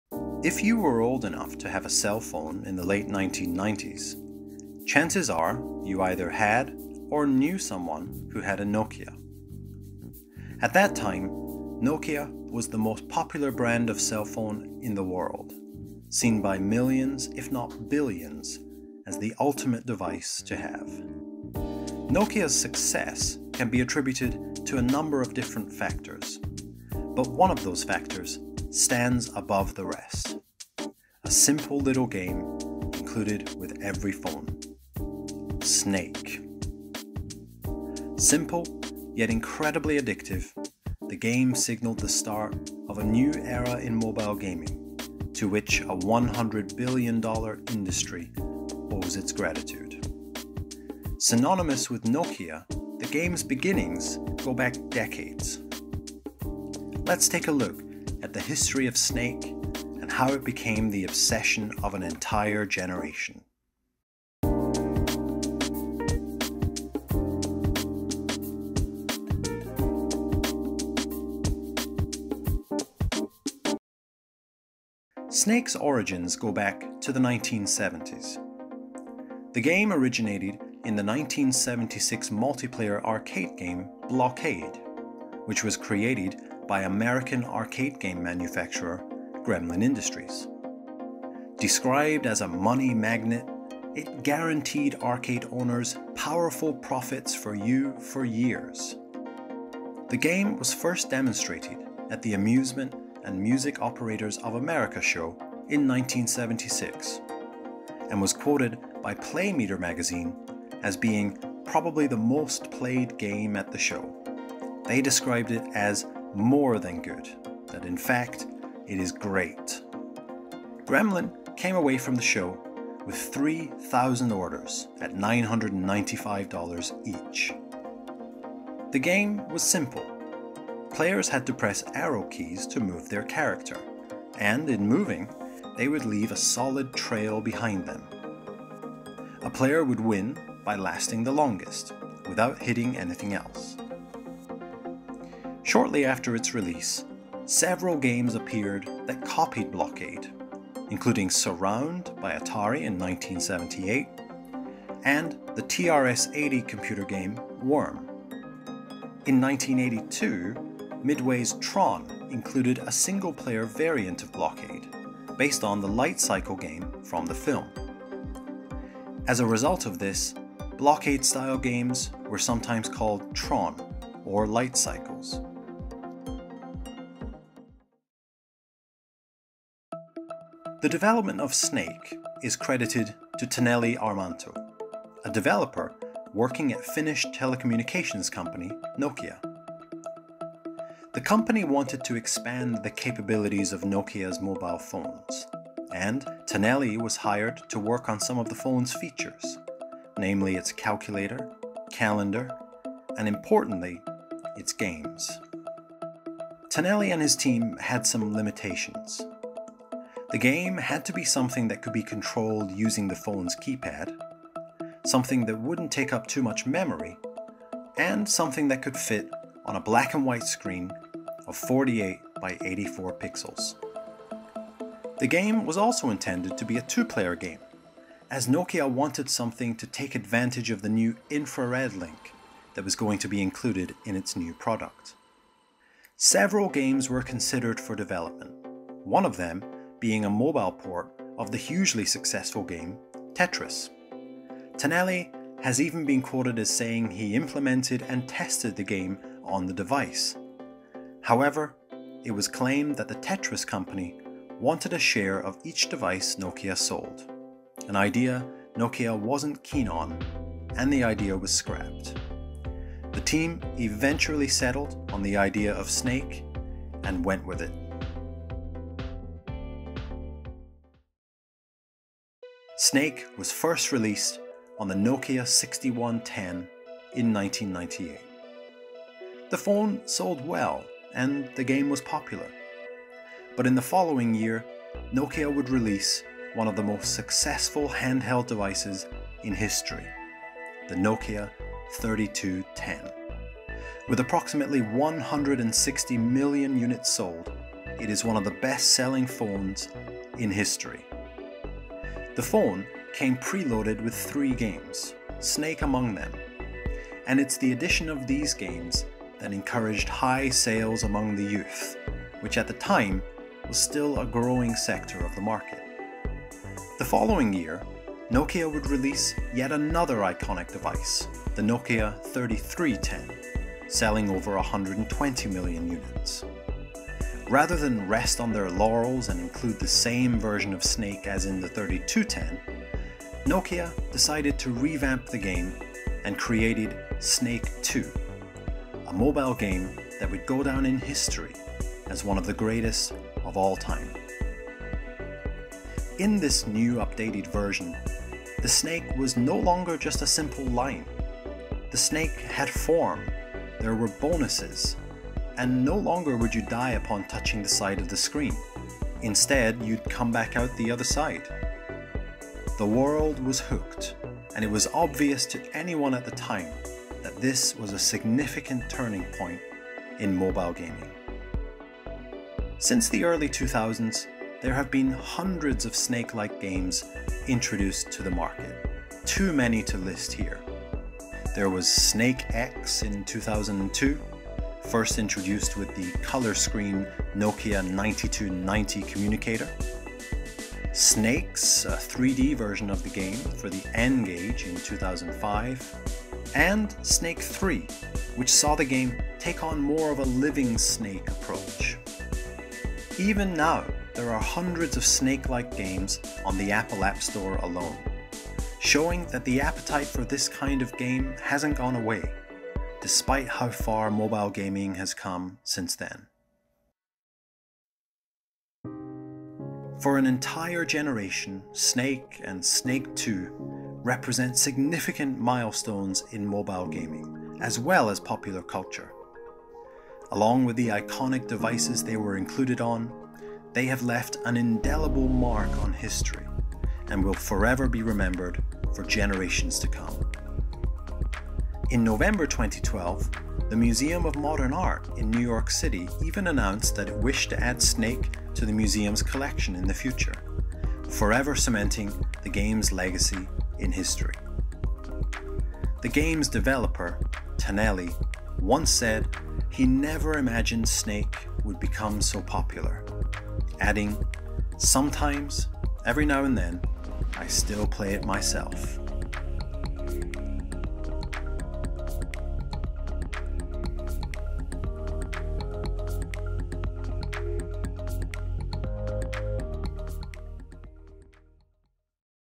The Making of Snake | A Short Documentary
Snake was first launched with the Nokia 6110 in 1998, and it marked a significant milestone in the development of mobile gaming.  A game remembered fondly by those who grew up in the late 90s and early 00s.  Join us as we take a look at the history and development of Nokia's classic game, Snake.